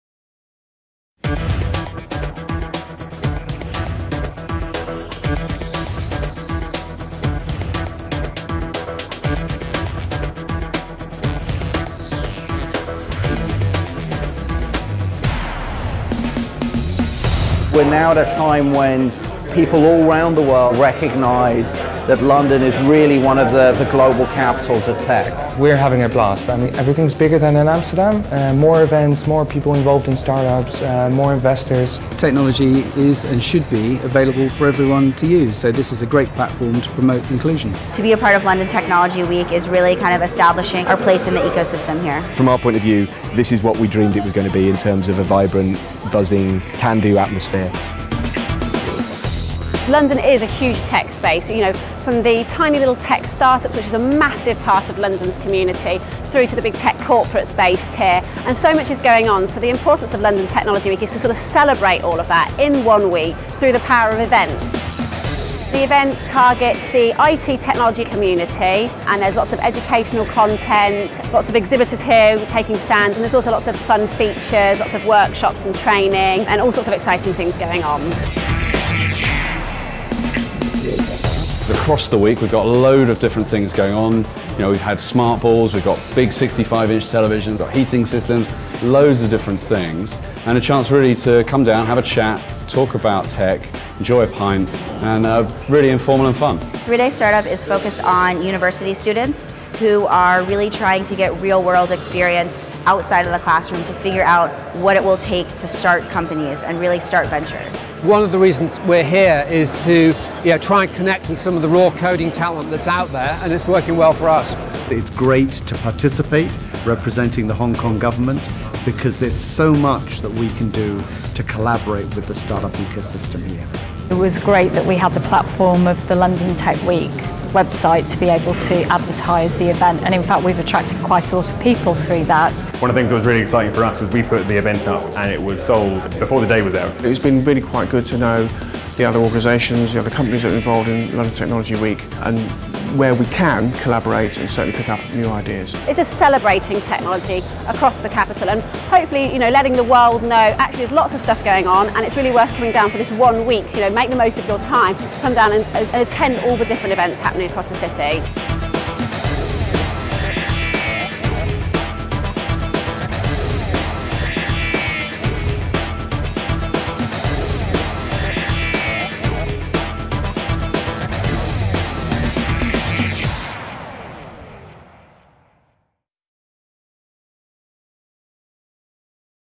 London Technology Week 2014
The inaugural London Technology Week took place in June 2014 and showcased London’s outstanding tech credentials. More than 200 events combined to host tens of thousands of tech experts from across the globe, covering everything from robotics to big data, wearable tech to fintech, and so much more.

to find out more